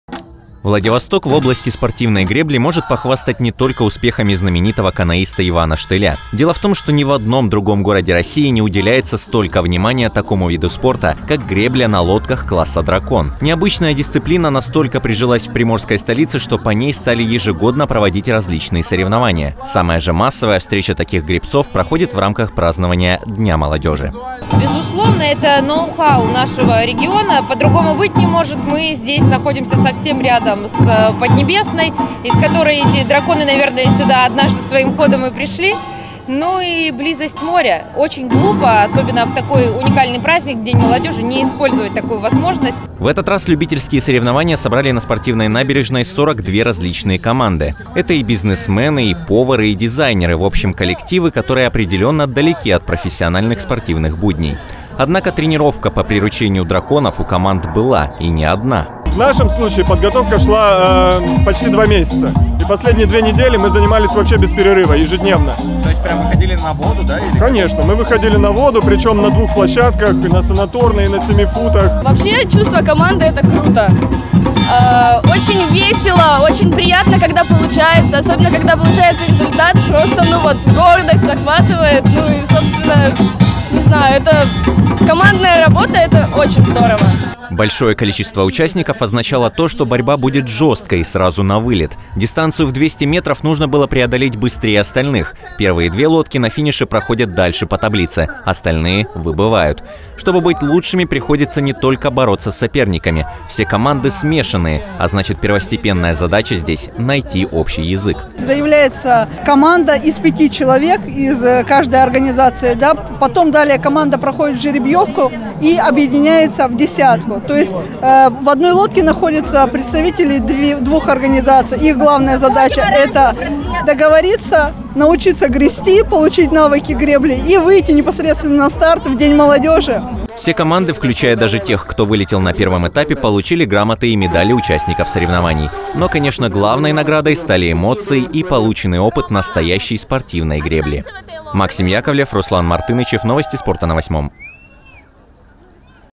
Во Владивостоке прошли любительские соревнования по гребле на лодках класса «дракон»
Гонки на лодках класса «Дракон» провели в краевом центре в рамках празднования Дня молодежи. Навыки в командной гребле показали более 40 коллективов. Все участники – любители. Менеджеры, повара и представители других профессий преодолели дистанцию в 200 метров.